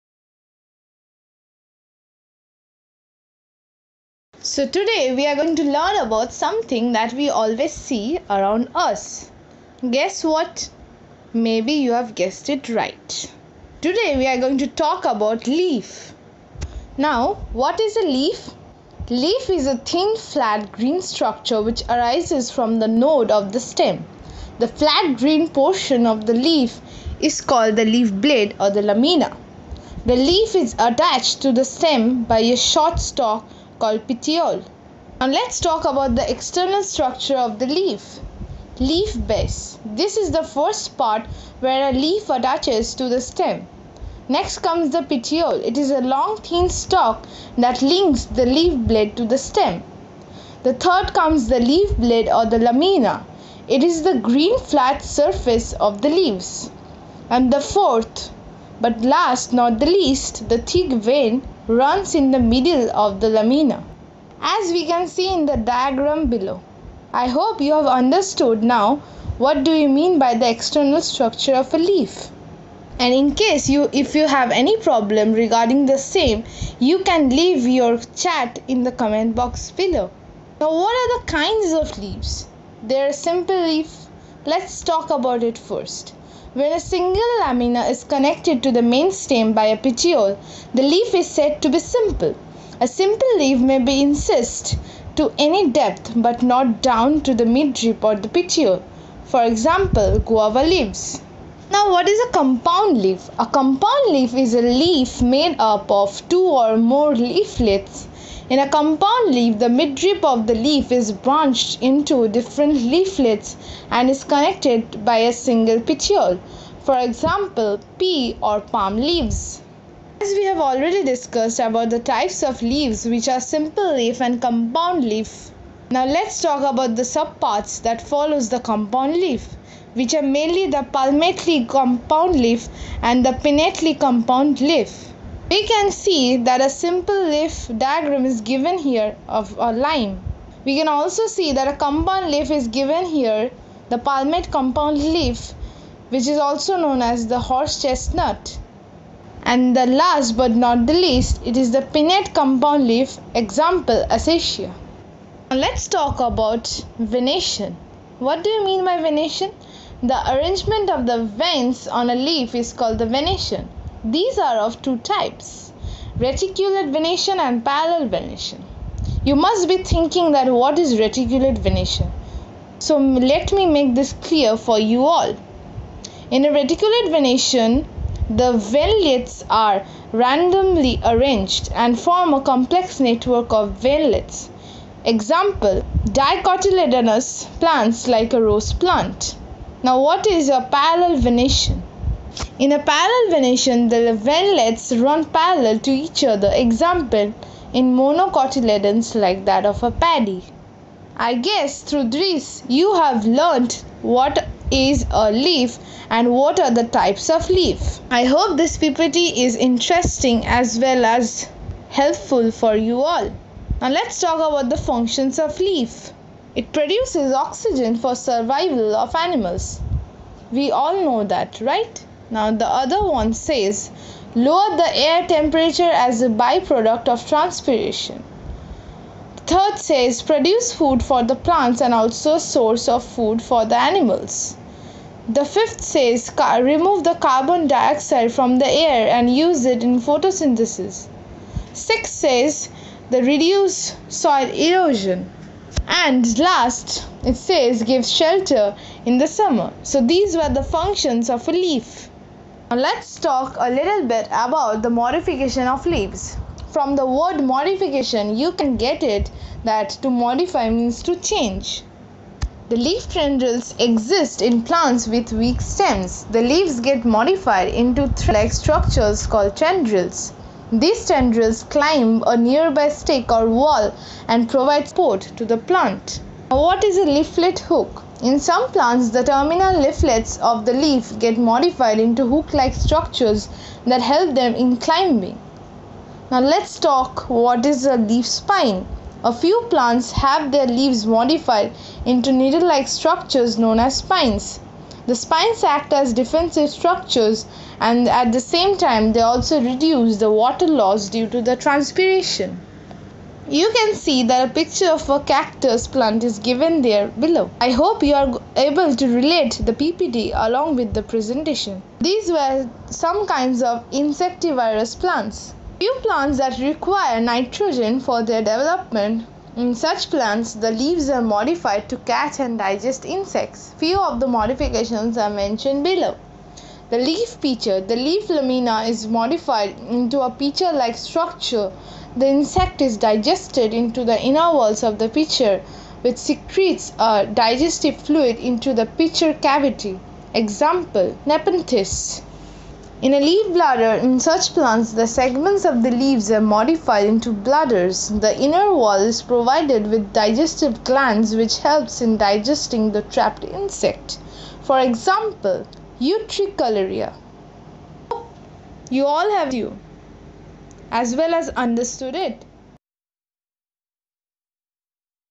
Leaf for Class VI,ICSE by Sweta Bhowmick
Leaves, usually, are thin and flattened organs, borne above ground.
There are varieties of leaves in terms of shapes, sizes, and textures. Likewise, different species of plants have different shapes, sizes, and textures of leaves.
Editor-Rajesh Panda,Dubbing & Technical Assistance-Namrata Guhathakurta, under Ankita Chakraborty,The technical Head of the team.